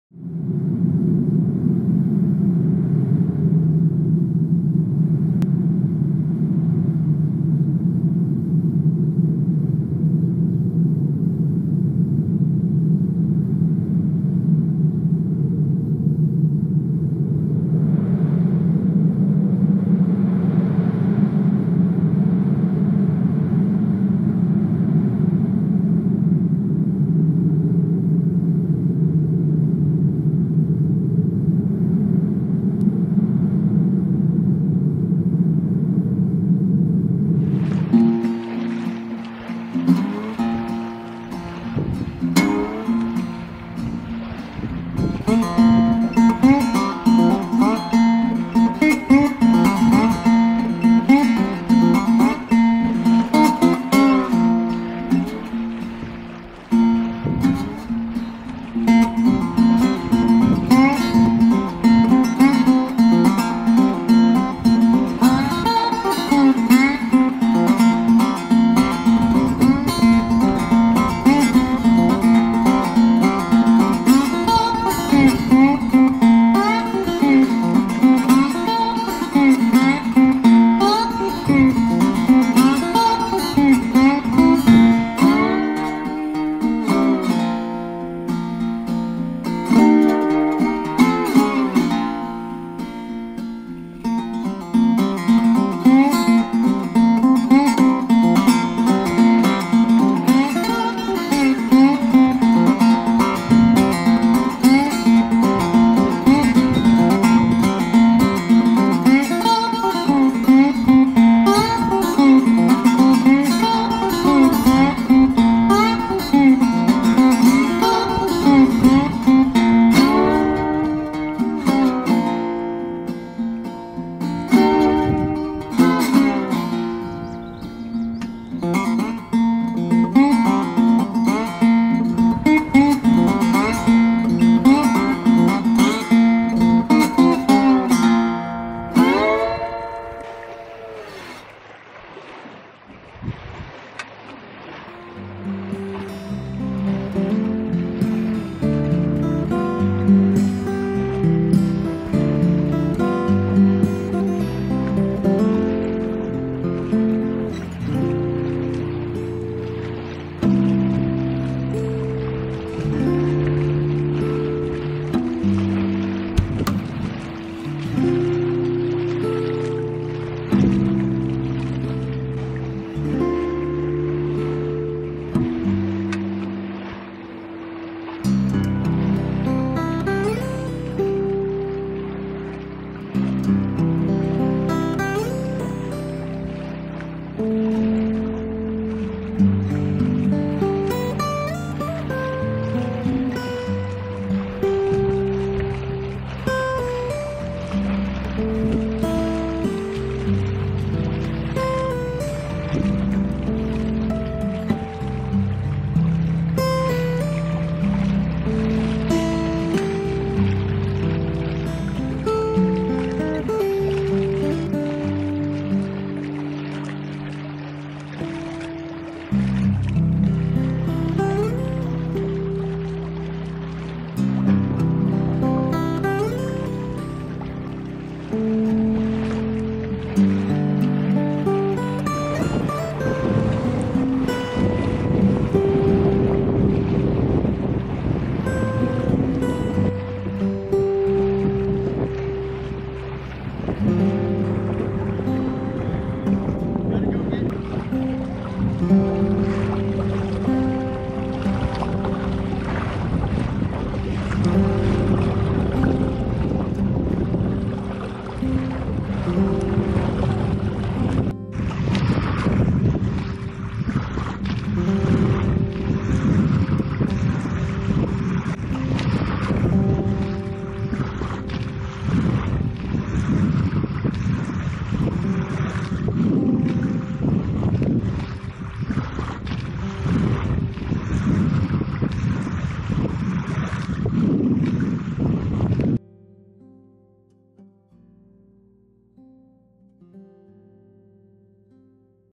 Union Valley Sail June 2018 Version 2
A nice sail in my Holder 17 on Union Valley Reservoir, Northern Caliornia, in June 2018. Includes a couple of short clips of members of the Potter Yachters shot later that month.